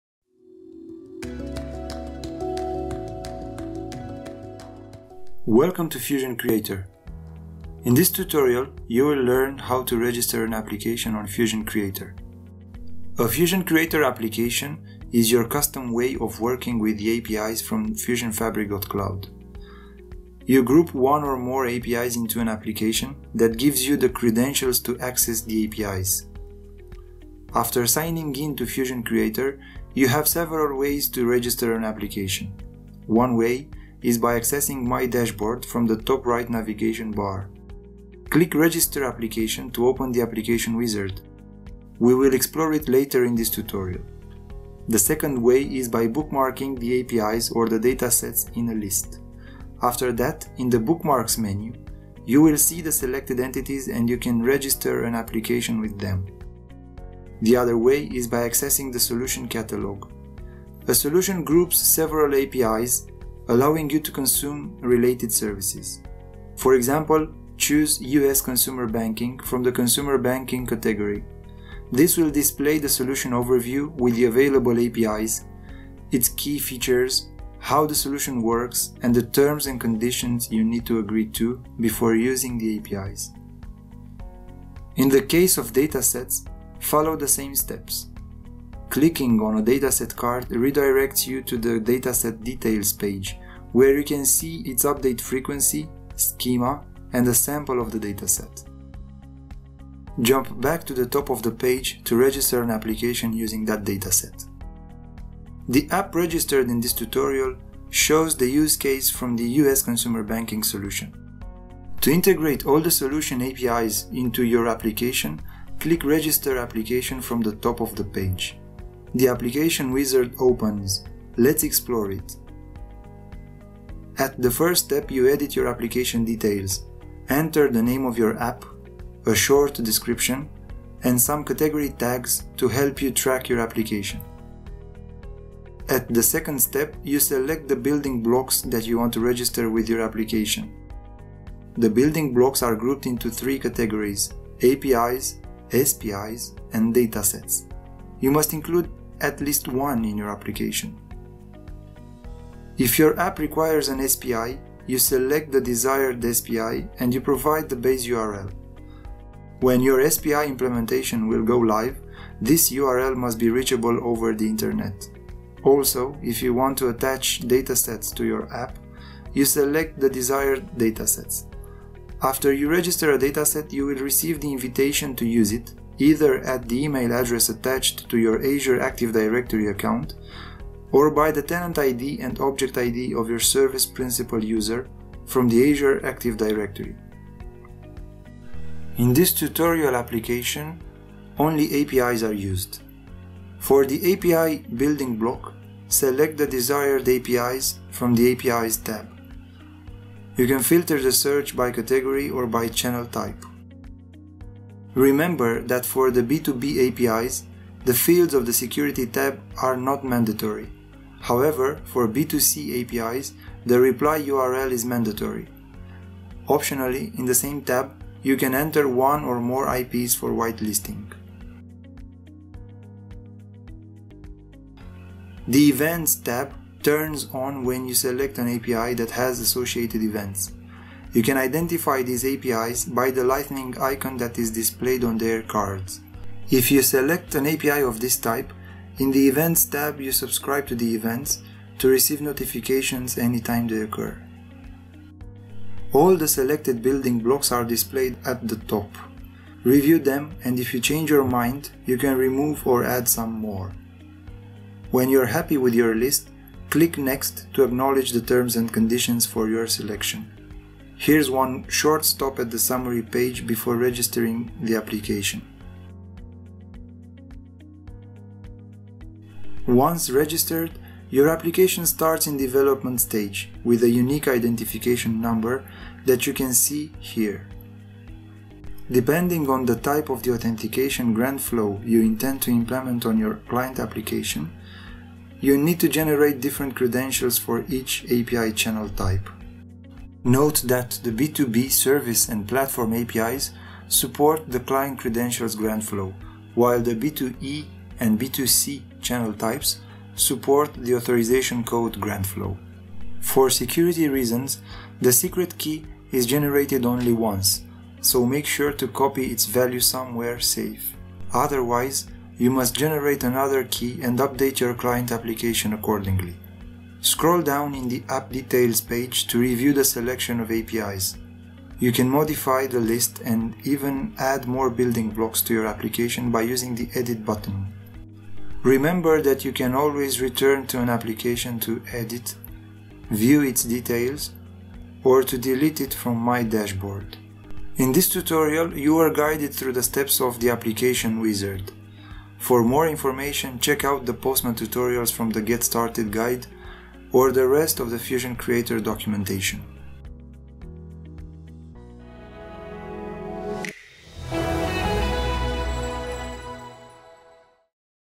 FusionCreator – Build an Application
In this tutorial you are guided through the steps of the Application Wizard on FusionCreator.